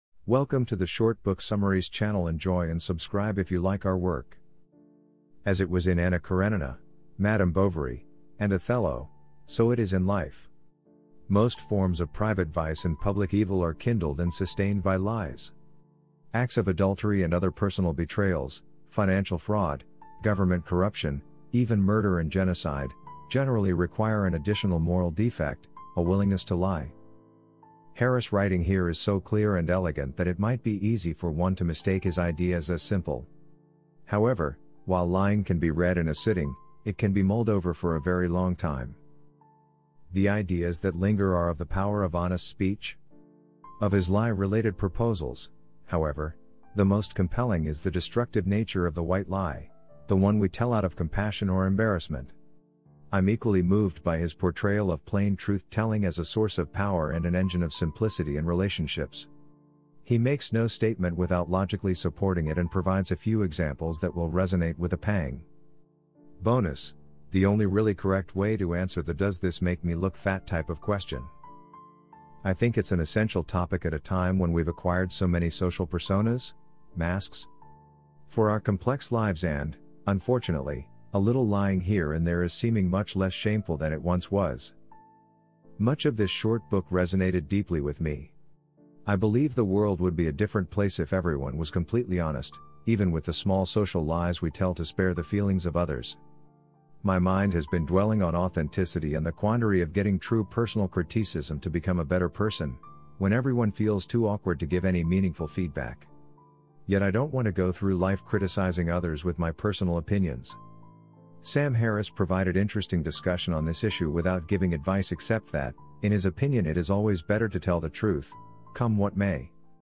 Short Book Summary of Lying by Sam Harris
As it was in Anna Karenina, Madame Bovary, and Othello, so it is in life. Most forms of private vice and public evil are kindled and sustained by lies. Acts of adultery and other personal betrayals, financial fraud, government corruption—even murder and genocide—generally require an additional moral defect: a willingness to lie.

Harris' writing here is so clear and elegant that it might be easy for one to mistake his ideas as simple. However, while 'Lying' can be read in a sitting, it can be mulled over for a very long time.

The ideas that linger are of the power of honest speech. Of his lie-related proposals, however, the most compelling is the destructive nature of the white lie; the one we tell out of compassion or embarrassment. I'm equally moved by his portrayal of plain truth-telling as a "source of power and an engine of simplicity" in relationships. He makes no statement without logically supporting it and provides a few examples that will resonate with a pang. (Bonus: the only really correct way to answer the "does this make me look fat" type of question)

I think it's an essential topic at a time when we've acquired so many social personas (masks?) for our complex lives and, unfortunately, a little lying here and there is seeming much less shameful than it once was.

Much of this short book resonated deeply with me. I believe the world would be a different place if everyone was completely honest, even with the small social lies we tell to spare the feelings of others. My mind has been dwelling on authenticity and the quandary of getting true personal critisism to become a better person, when everyone feels too awkward to give any meaningful feedback. Yet I don't want to go through life criticizing others with my personal opinions. Sam Harris provided interesting discussion on this issue without giving advice except that, in his opinion it is always better to tell the truth, come what may.